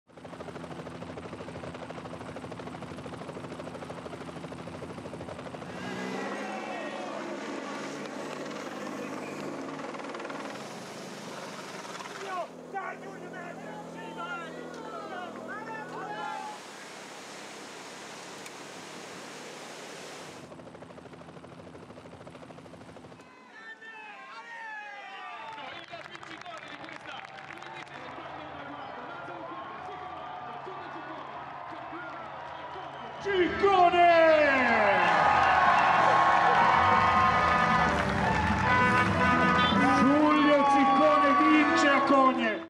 Rinasce Giulio Ciccone!Terza vittoria italiana al giro 2022
L' abruzzese della Trek-Segafredo  vince di forza e classe il tappone alpino di Cogne, conquistando il suo terzo successo al Giro.⁠

Il Negozio di GCN Italia: [REPLACE]
GCN Italia T-Shirt: [REPLACE]